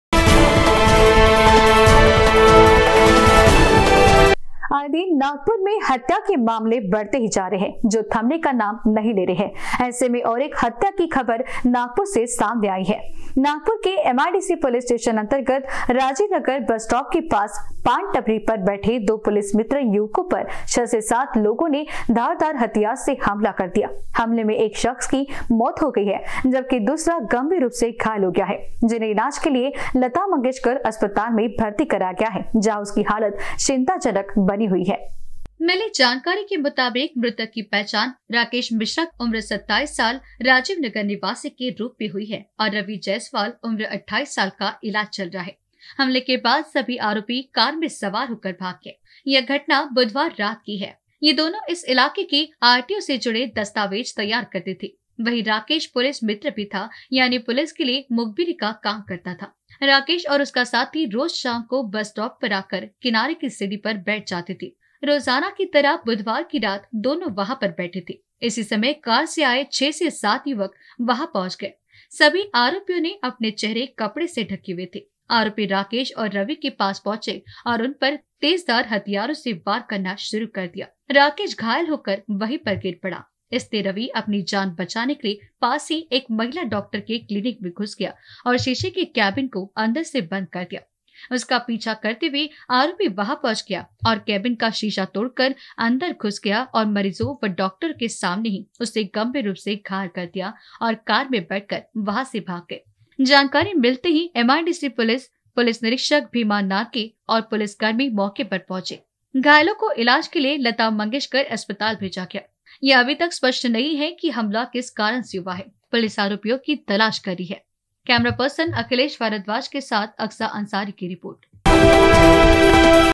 दो पुलिस मित्रो पर सात लोगों ने किया घातक हमला.
दो पुलिसकर्मियों पर सात लोगों ने किया घातक हमला.